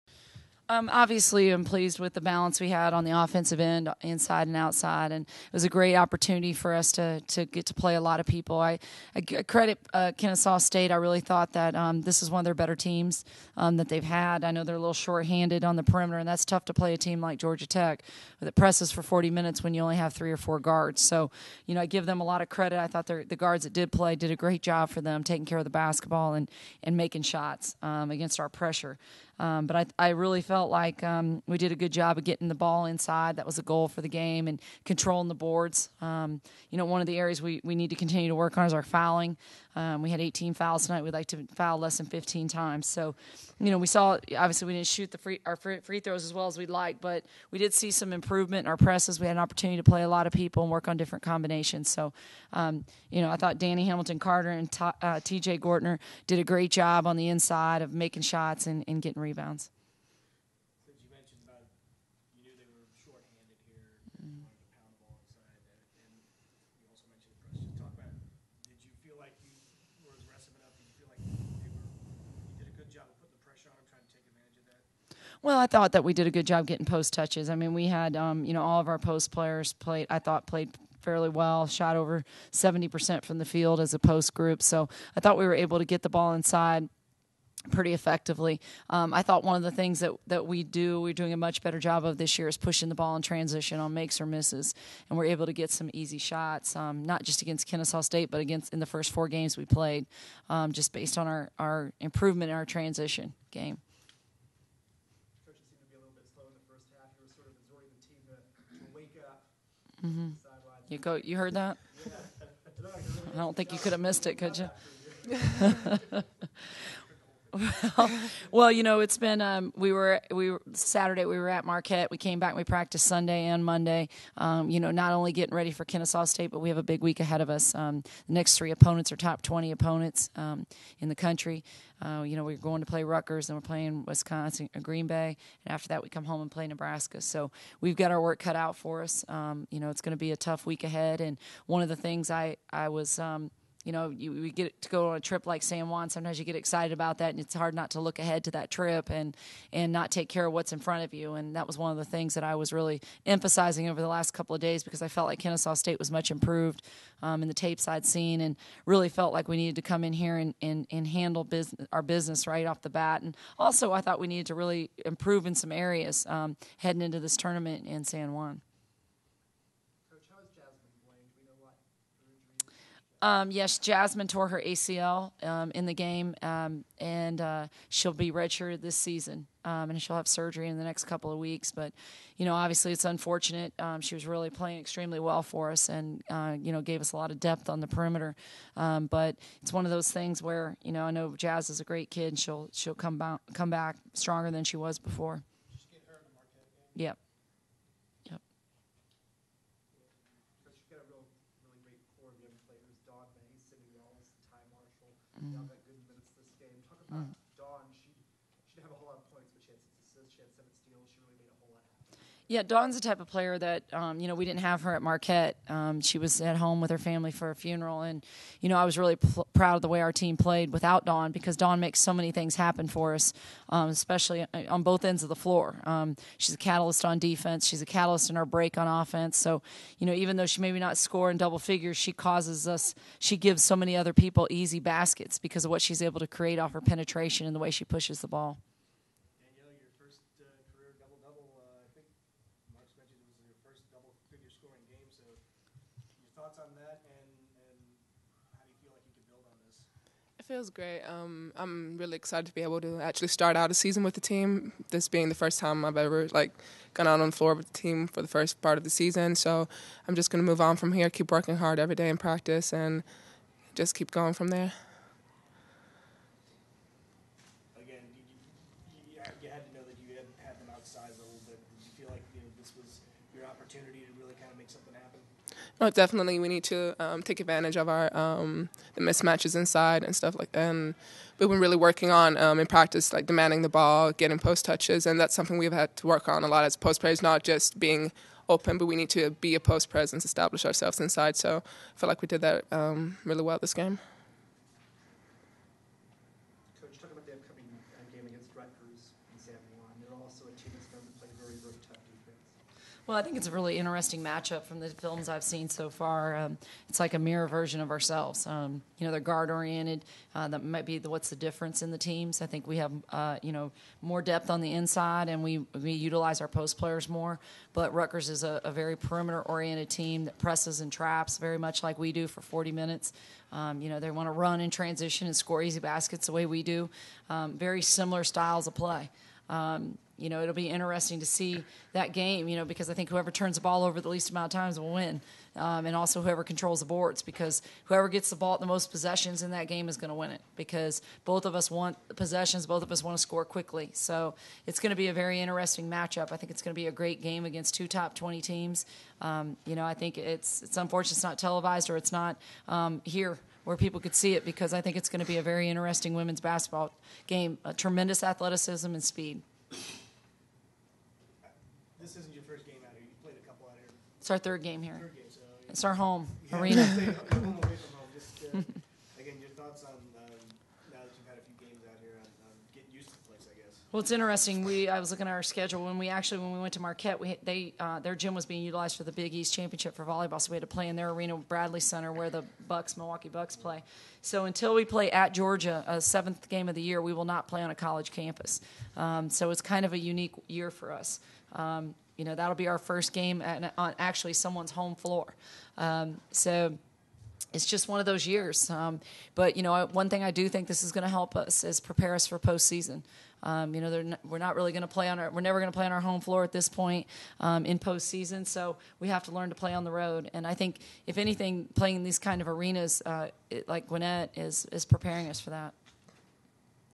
Georgia Tech Women's Basketball Press Conference - 11/22/11
The Georgia Tech Women's Basketball team defeated Kennesaw State, 94-51, Tuesday night at the Arena at Gwinnett Center - Coach Joseph and Danielle Hamilton-Carter met with the media.